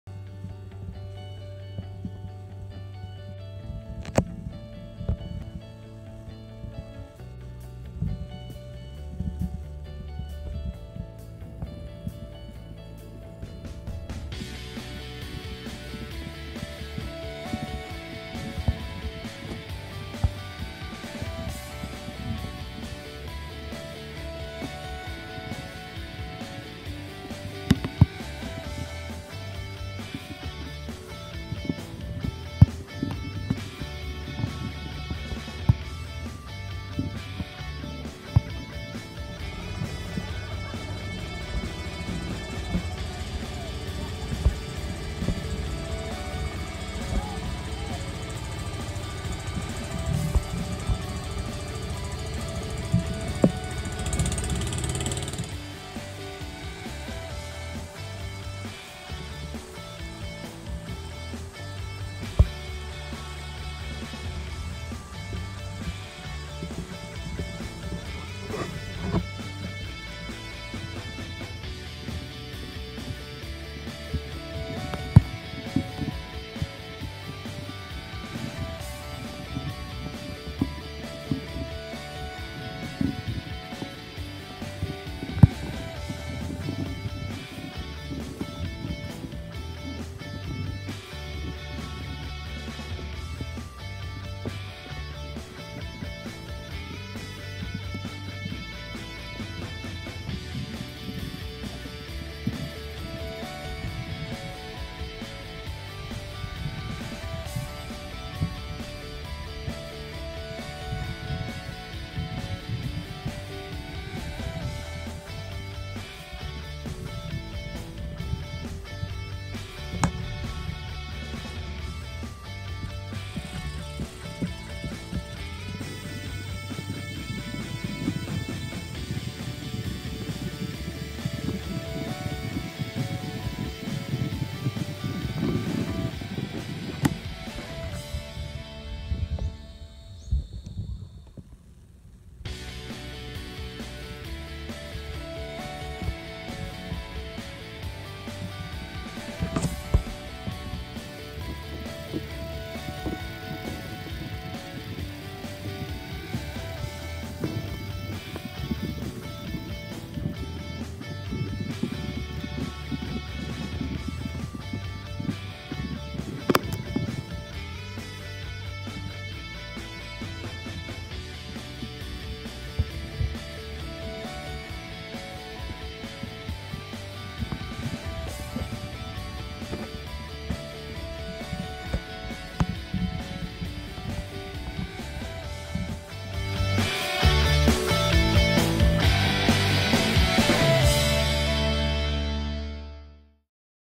Surigao Bike Trail: Kinabutan River to Mana Pepang Village - SECRET TRAIL! 😱 SURIGAO CITY! 🚴🏻💪😊
Kinabutan Riverbank to Mana Pepang Village Secret Trail 😱 💪 🚲 🔥

Recovery Ride Daya Na Explore2x 😅

Also basang-basa sa ulan ride 🌧 ☔️

Na curious dajon ako... amo tag explore sab nako to find out for myself how’s the road / trail condition going to Mana Pepang ky waya may pictures / videos ako nakit-an miski isa kabook sa mga naka-agi na... Interesting...✌️

At first I was a bit doubtful na basi lisod ini na trail, arang ka sagbot or arang kalapok... but then I was surprised na well stablished naman baja sija... just waiting for it to be discovered rakan hehehe charbaki 🐸

kinabutan bridge
Sitio kinabutan
Kinabutan flood wall
Kinabutan river wall surigao
Kinabutan trail
Mana pepang trail